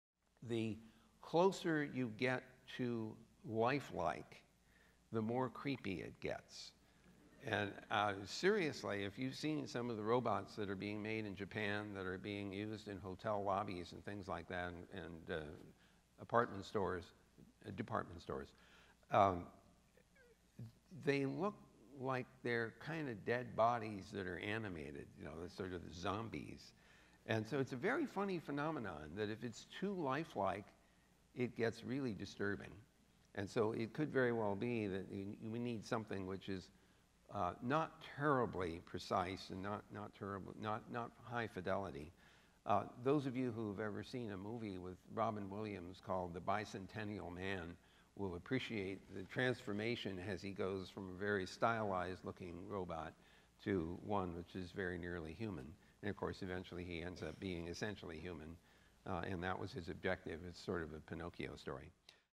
Cerf says some humaniform robots look "creepy"
He says the look of newly developed service robots used in hotels in Japan is somewhat zombie-like when the aim is to make them appear lifelike.